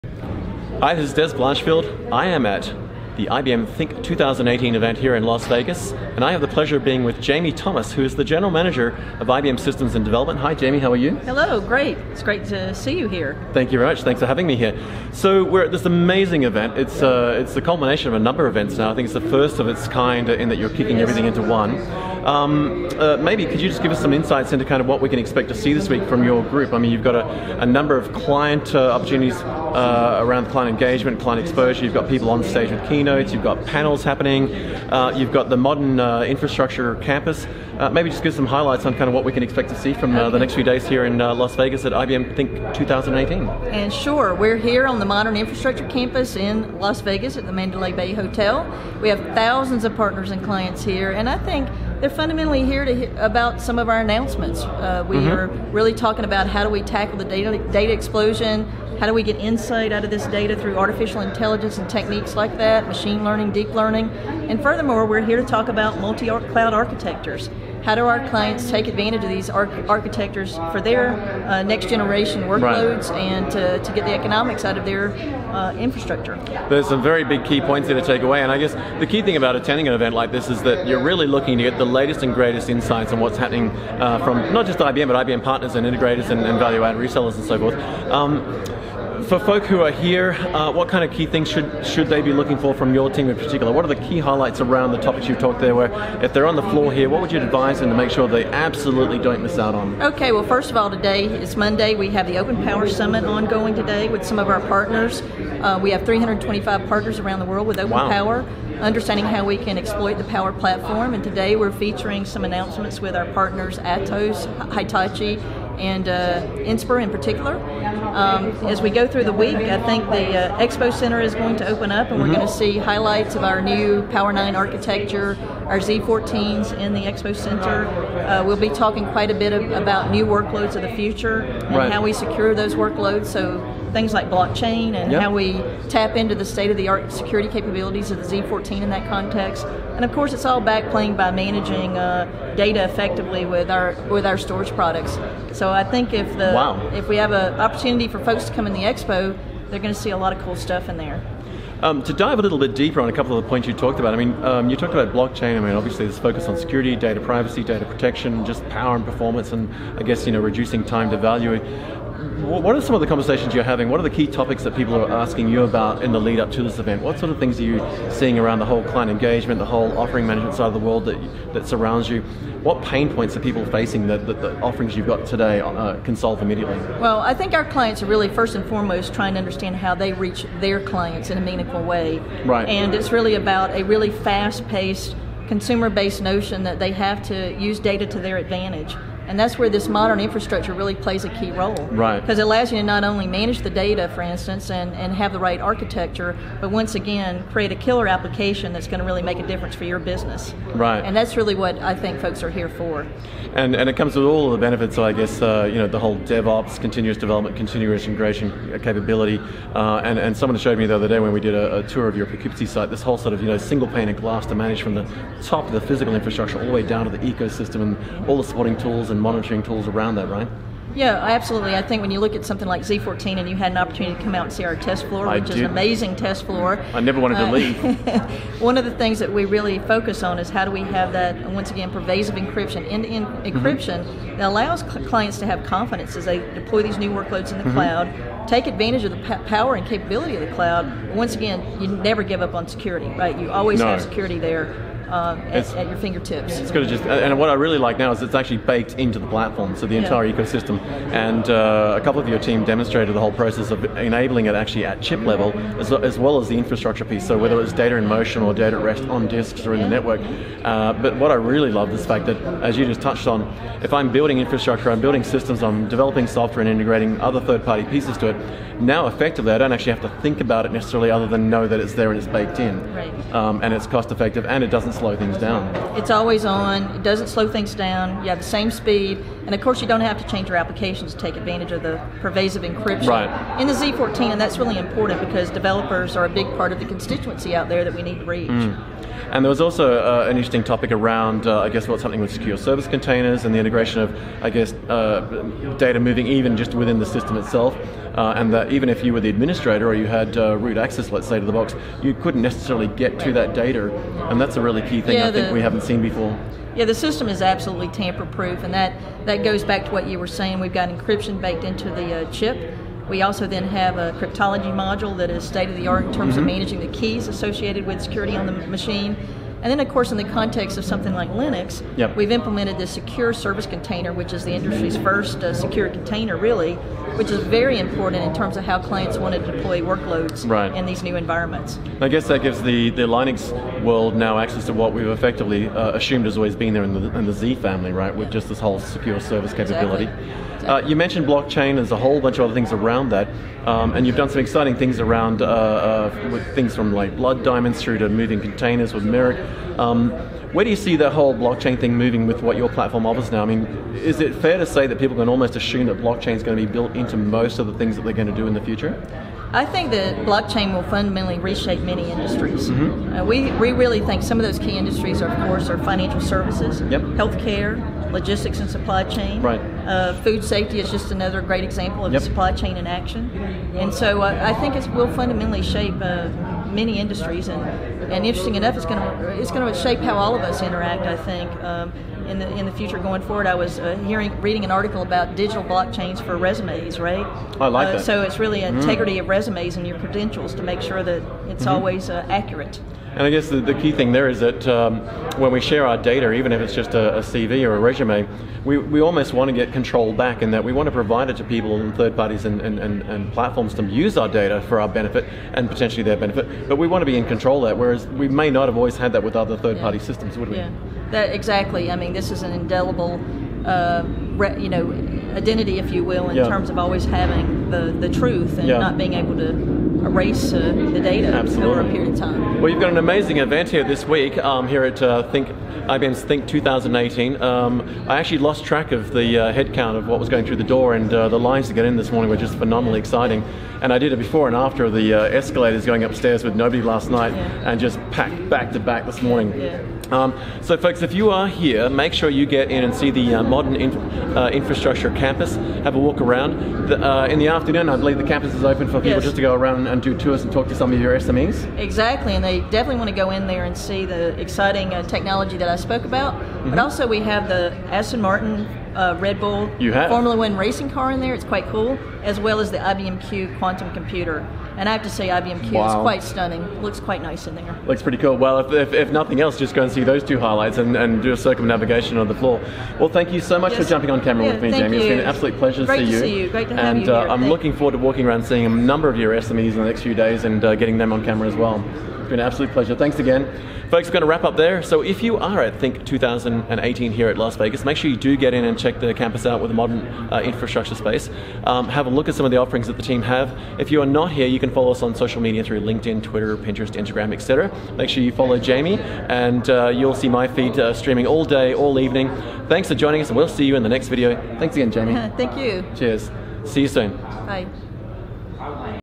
"THINK2018 - Talking With Jamie Thomas about IBM Multi Cloud"
Hi, I am Dez Blanchfield, and I had the chance to speak with Jamie Thomas from IBM at the THINK 2018 event in Las Vegas. Jamie is the General Manager of IBM’s systems and development. We talked about the multi cloud, infrastructure campus, big data, and much more. Jamie talked about how they have thousands of their clients present at the event to listen to some of the big announcements IBM has, like tackling data explosion, getting inside out of the data through artificial intelligence (AI), machine learning (ML), deep learning, multi cloud architectures and how do organisations take advantage of it for future workload, and more.

Jamie shared her insights on the key highlights of the event that IBM has for the people, which included the open power summit, a sneak peek of the new power nine architecture, Z 14, how to manage data effectively, blockchain, and some other amazing things as well. I asked Jamie to talk about the key points they are being asked by the audience. She spoke about their clients trying to understand an effective way to reach out to their clients for a better customer experience. She also talked about how they focus on pervasive encryption, which allows the clients to have confidence as they take advantage of the cloud. Jamie shared her views on how she thinks blockchain will reshape various industries like finance, healthcare, logistics, etc.

IBM’s THINK2018 was a great success, and I witnessed a lot of people as I made a before and after video capturing very few to no people the previous night to a full house today. The modern infrastructure campus was a must visit as people could go there and talk about the various technologies with SMEs from IBM.

Please plug in your earphones and listen to what Jamie Thomas has to say about quantum computing at IBM, the highlights from the event, and more.